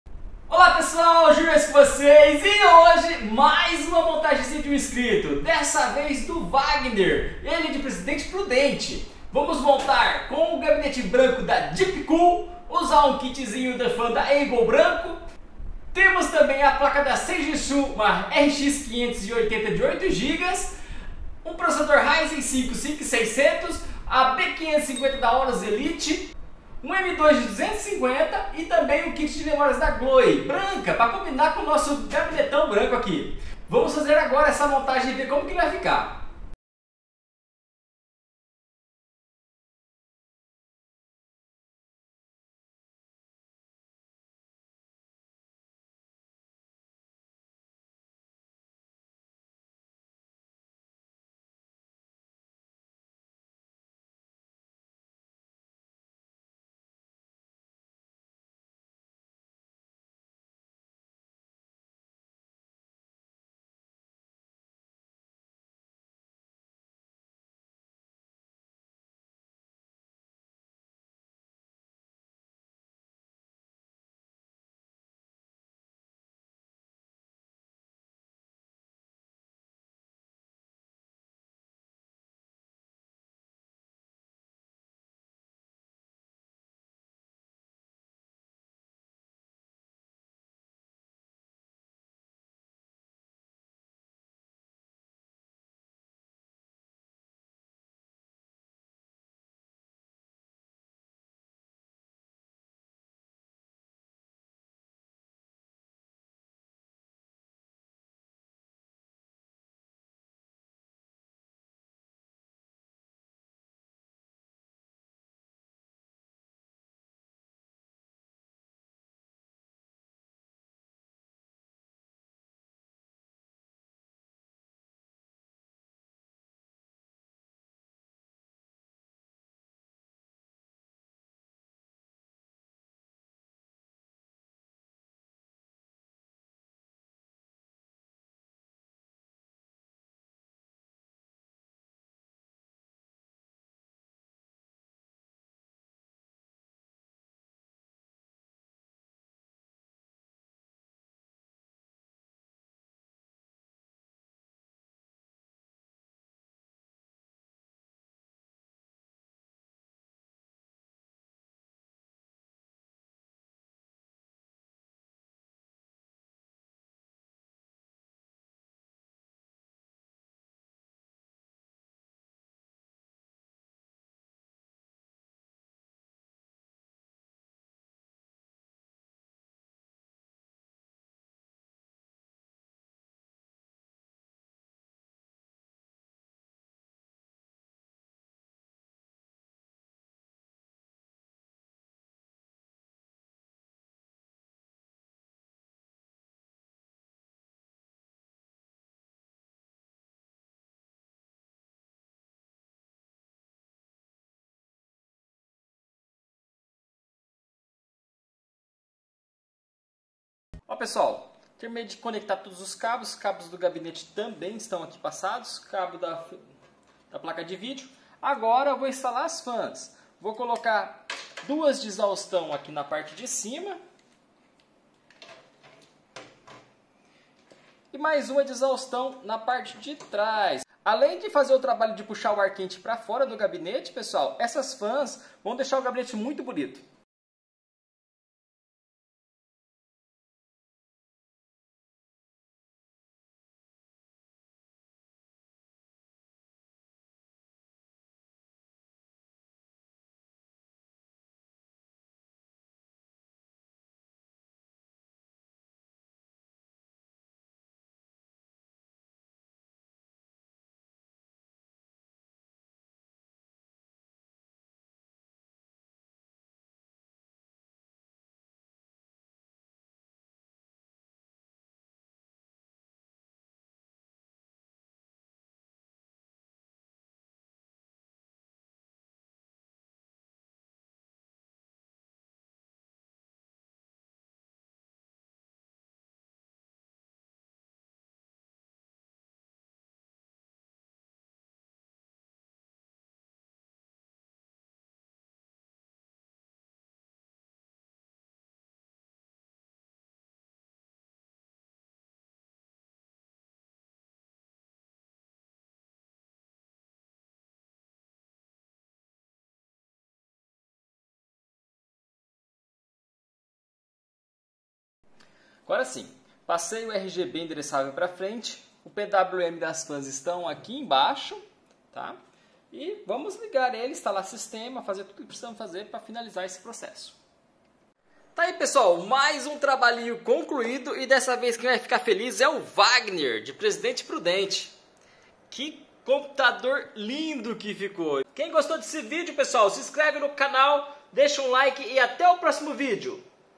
MONTAGEM PC GAMER COMPLETO DE UM INSCRITO DO CANAL! - GABINETE GAMER DEEPCOOL, B550, Ryzen 5 6000
GRÁTIS – AJUDO VOCÊ A ESCOLHER AS PEÇAS DO SEU PC, ME CHAME NO WHATS: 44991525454

PLANILHA COMPLETA DE LINKS ALIEXPRESS!